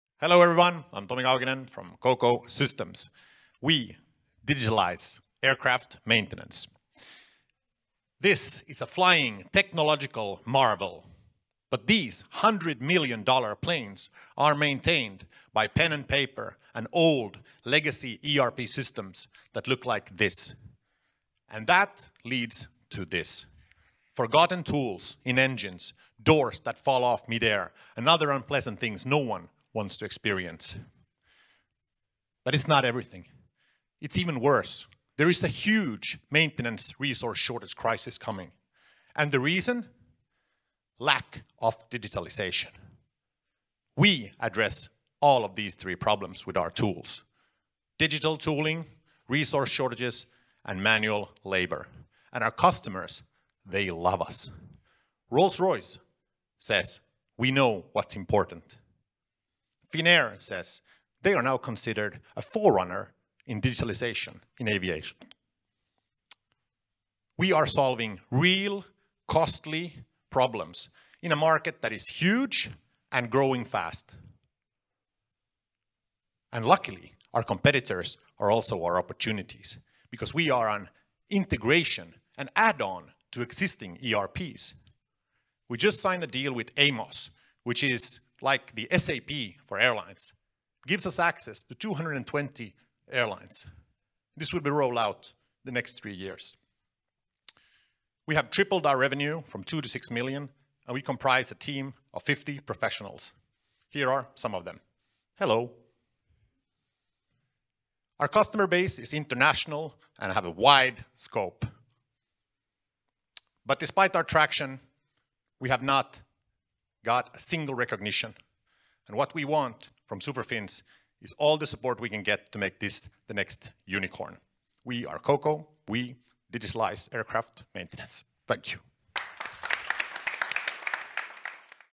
QOCO Systems' pitch @ SUPERFINNS 2024
Welcome to the SUPERFINNS final event! We wanted to present the top startups and their pitches, selected from the SUPERFINNS accelerator program. Each video features a 2-minute pitch by innovative entrepreneurs showcasing their groundbreaking ideas.

Held at the vibrant Maria01 in Helsinki, this event (along with the whole program) is organized by Kasvu Open in collaboration with Nordea's Startup & Growth unit.